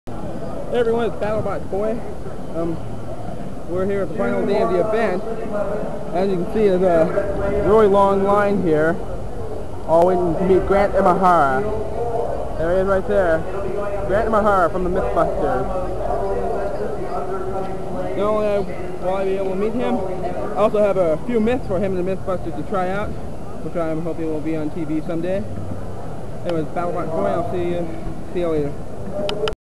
RG10-Waiting to meet Grant Imahara
Me & my mom are waiting in line to meet Grant Imahara himself.